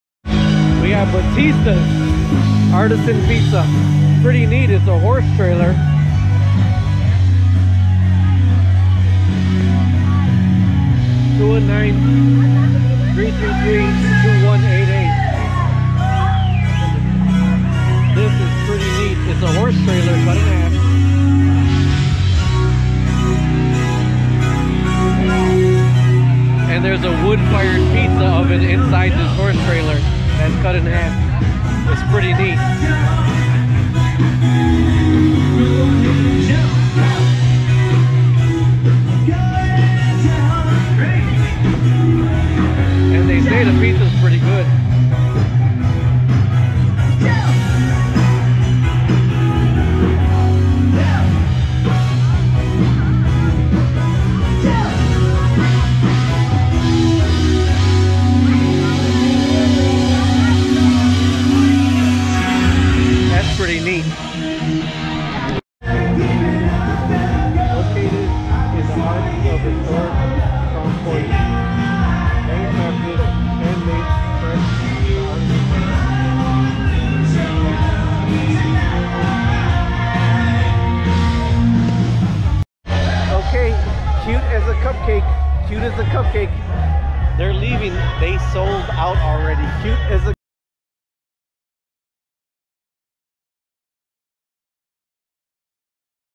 Battistas artisan pizza MOBILE pizza trailer Hammond Venetian night
What's going on everybody Rob here with a little video on this woodfired pizza trailer. Yes I said a woodfired pizza trailer. This gentleman has a woodfired pizza oven inside of a horse trailer. He modified a horse trailer to make it a woodfired pizza oven. The pizzas are handcrafted artisan pizzas. I guess the main pizza place he owns is out of Crownpoint Indiana and he had this woodfired pizza trailer or woodfired pizza oven at the Hammond Indiana Venetian night. I took down the number because I don't know if they do parties also but it would be awesome if you had a party to had to have these hand tossed artisan pizzas at your party you know kind of like when they have the Tyco vendor or sub sandwiches. To have a hand fired a woodfired pizza oven at your party. It's called Batista's pizza. I believe there out of Crownpoint Indiana I think they have a restaurant there if I read correctly on the side of the horse trailer. While on the side of the wood fired pizza oven LOL. These pizza from this Batista's pizza horse trailer smelled amazing this is an excellent idea and it was a great project to make this horse trailer into a woodfired pizza oven.

For watching,

Rob here a.k.a.Trev2323